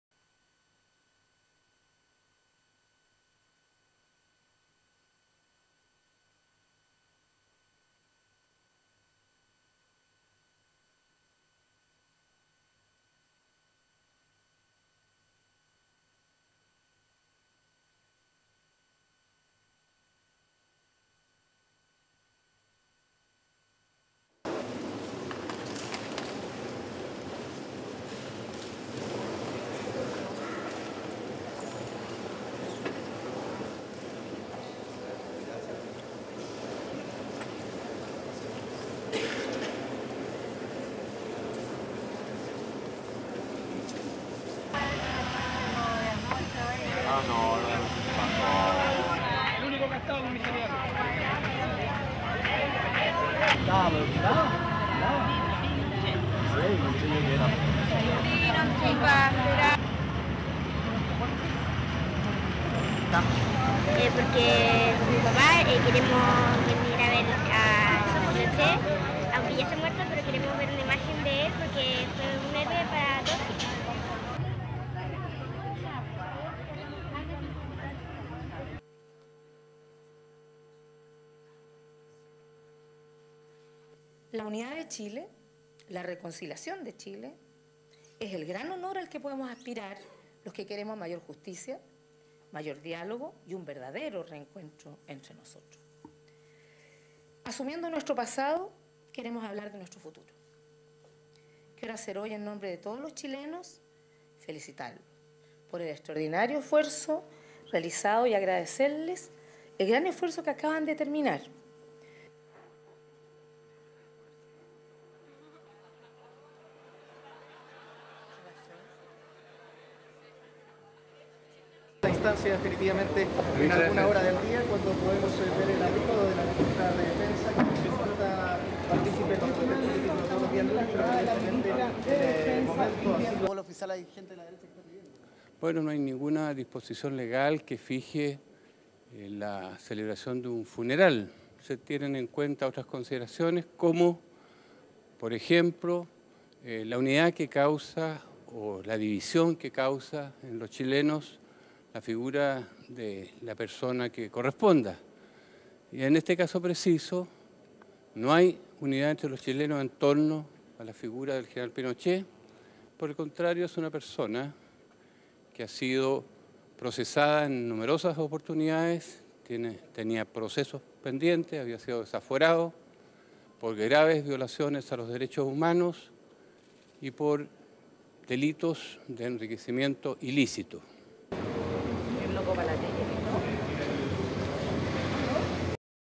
WRAP President, Interior min on Pinochet's death, Defence minister pay respects
++Shots 1-3 are Mute++
1. Chilean President Michelle Bachelet inspecting guard at La Moneda presidential palace
2. Top shot of former Chilean military dictator General Augusto Pinochet's body
3. Pinochet's youngest son Marco Antonio
4. Pinochet in coffin
5. Close-up of Pinochet
6. Close-up of Lucia, Pinochet's ex wife, embracing woman
7. Various shots of people lined up outside Military Academy
8. People holding Chilean flag
9. SOUNDBITE (Spanish): Vox Pop, Maria Graciela, resident
"My dad brought me and also because this is a very important man in history."
10. People lined up outside academy
11. Chilean President Michelle Bachelet greeting officials
12. SOUNDBITE: (Spanish) Michelle Bachelet, Chilean President
"The unity of Chile, the reconciliation of Chile is the biggest honour we can hope for if we want more justice, more dialogue, and more unity aware of our past. We want to think of our future. In the name of the Chilean people, I want to thank you all for all the work you have just done."
13. More of Bachelet greeting officials
14. Chilean Defence Minister Vivianne Blanlot entering ministerial building
15. SOUNDBITE: (Spanish) Belisario Velasquez, Chilean Interior Minister
"There is no legal position regarding a (state) funeral. What we have to take into consideration is the division that this person causes among the Chilean people. There is no unity among the Chilean people regarding General (Augusto) Pinochet. On the contrary he is a person who has been charged with human rights abuses and illegal gains."
16. Security around ministerial building
STORYLINE:
Chilean President Michelle Bachelet called for unity and reconciliation for Chile on Monday, as people continued to line up outside the Military Academy in Santiago to pay their final respects to former military dictator General Augusto Pinochet.
"The unity of Chile, the reconciliation of Chile is the biggest honour we can hope for if we want more justice, more dialogue, and more unity aware of our past," Michelle Bachelet said.
"In the name of the Chilean people, I want to thank you all for all the work you have just done," she added.
Throughout Monday Chileans filed past the coffin of the former dictator at the Santiago Military Academy to pay their respects.
The coffin, part covered with a Chilean flag and Pinochet's military hat and sword on top if it, was placed in a large hall.
People filed past, some crying over the coffin, others looking at the former dictator's body through a glass shield.
Pinochet's children, including Marco Antonio, Veronica, and Lucia were present and greeted well-wishers.
Chile's government said that Pinochet would not receive the state funeral normally granted to former presidents, but only military honours at the military academy.
"What we have to take into consideration is the division that this person causes among the Chilean people," said interior minister Belisario Velasquez.
Some in Chile celebrated with champagne but for others there was no joy in his death, only the sense of a final injustice committed by the dictator who brutally ruled the country for 17 years.
Pinochet, a fierce anti-Communist who overthrew Chile's democratically-elected Marxist president Salvador Allende in a bloody coup launched on 11 September, 1973, ruled the nation with an iron fist for 17 years.